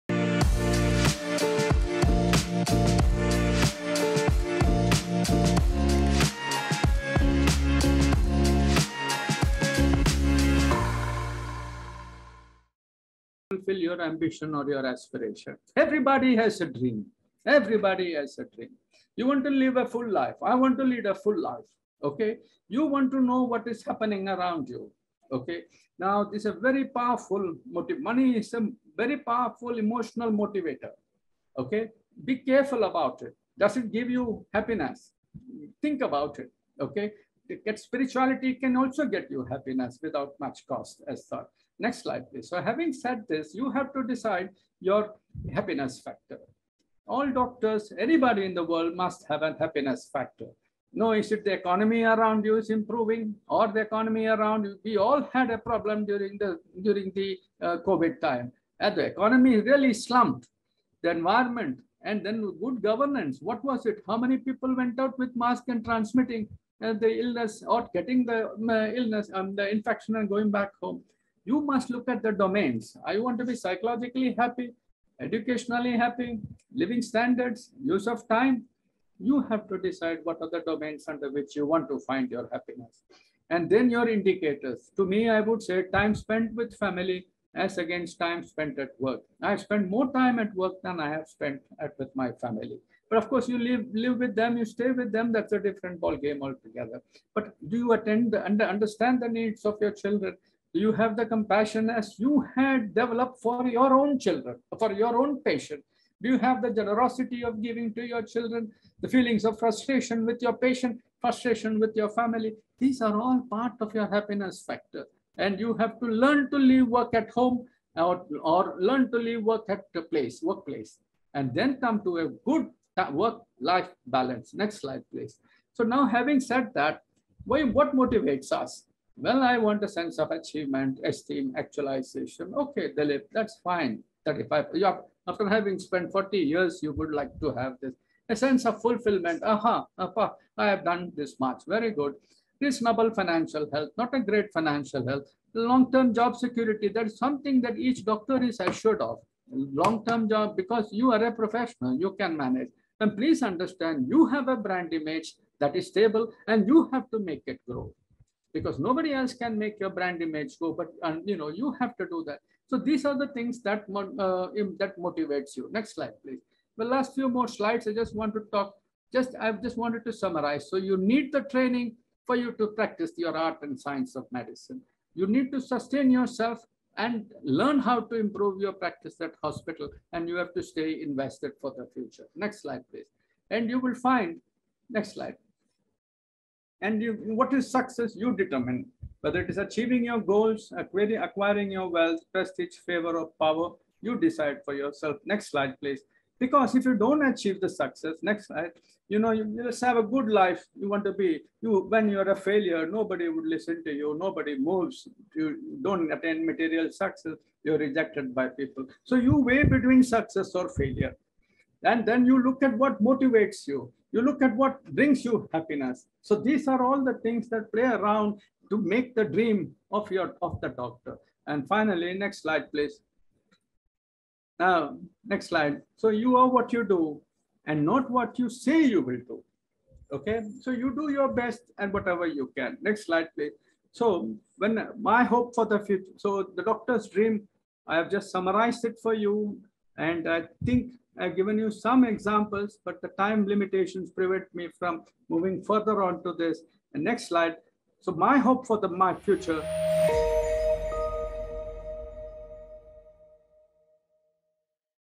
How to be Ambitious in Life
Ambition is a strong desire to do or achieve something in life. Ambition gives us aims, objects, goals, and targets of life. Once you become a doctor, you become a part of an elite community in the medical profession. Good doctors can improve the health system of a country making people better.

Dr. Dilip Mathai, Professor of Medicine and Adult Infectious Disease, Dean Apollo Institute of Medical Sciences & Research gives aspiring MBBS physicians a route to fulfill their aspirations and ambitions.